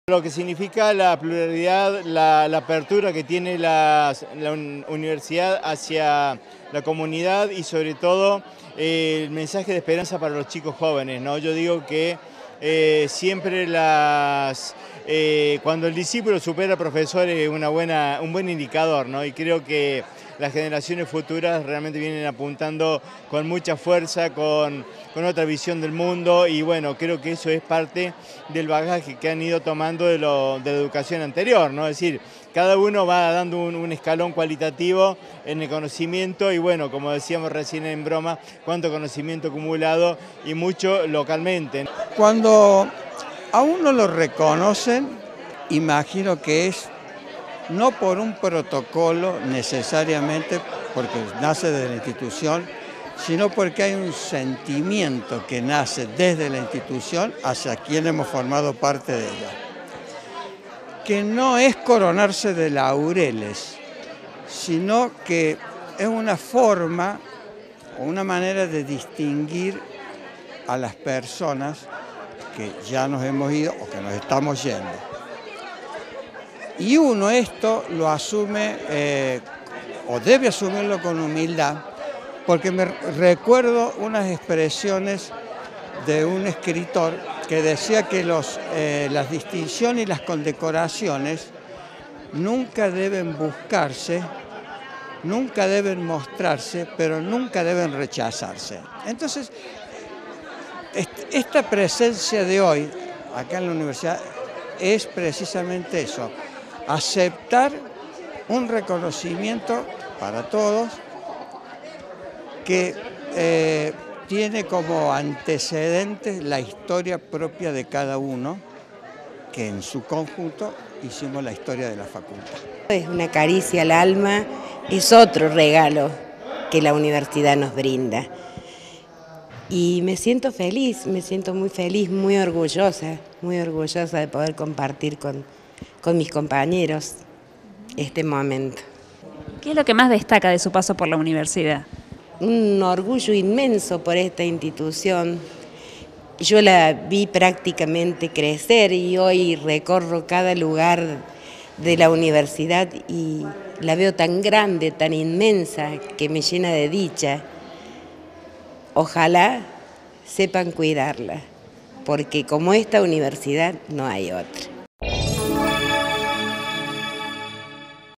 HOMENAJE A PERSONAL JUBILADO FAV
REALIZARON UN ACTO DE RECONOCIMIENTO A DOCENTES Y NODOCENTES DE LA FAV
Facebook.com/uniriotv
Twitter.com/uniriotv